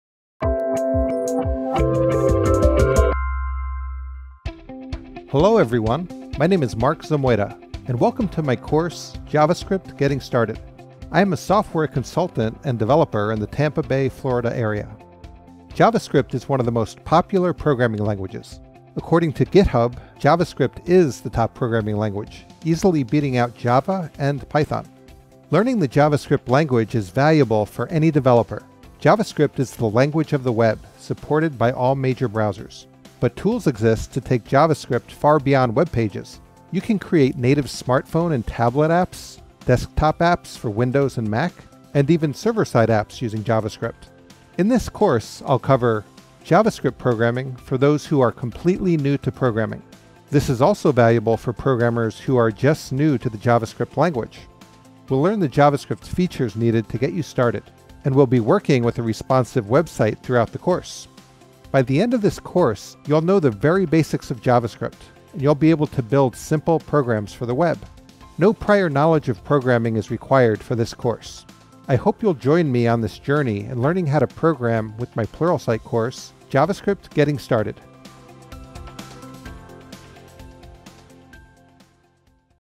JavaScript: Getting Started Course Preview
Join Pluralsight author Mark Zamoyta as they walk you through a preview of their "JavaScript: Getting Started" course found only on Pluralsight.com. Build your technology skills with Mark’s help by learning the types of applications that can be built with JavaScript, and the platforms they’ll run on.Next, you’ll explore the basics of JavaScript, learning from plenty of JavaScript examples. Lastly, you’ll put your JavaScript knowledge to work and modify a modern, responsive web page. Visit Pluralsight.com to start learning JavaScript skills today!